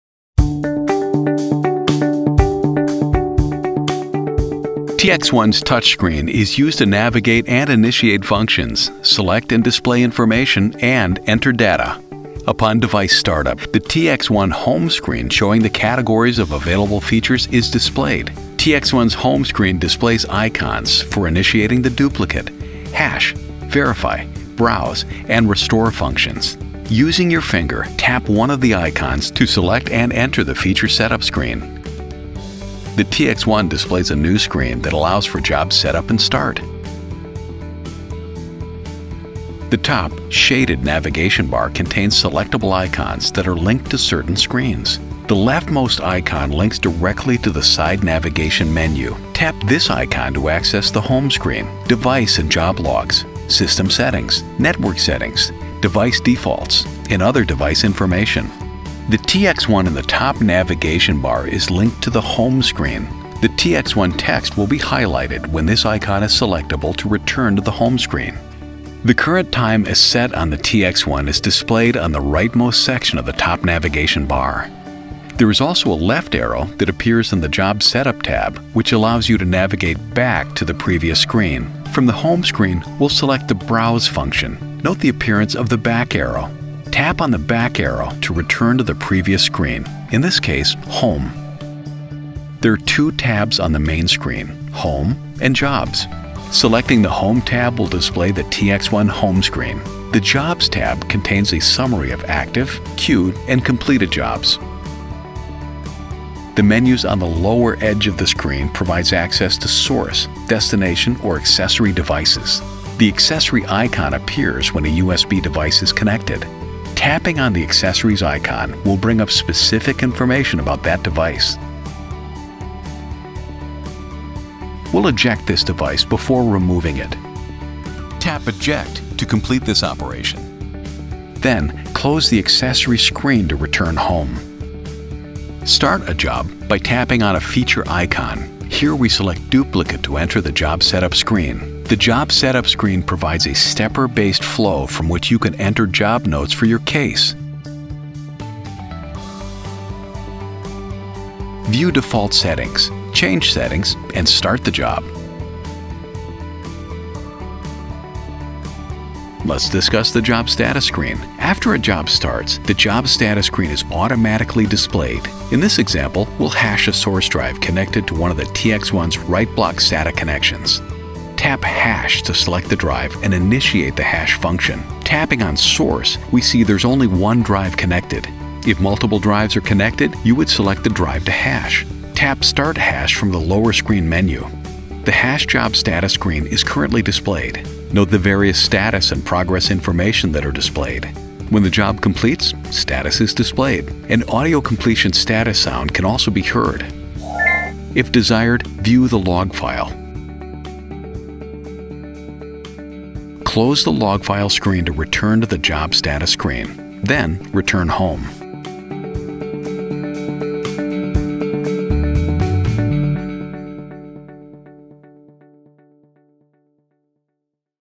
TX1 User Interface Overview
This overview shows how to use the TX1 Forensic Imager's touch-screen user interface for device control,  job management, and information access.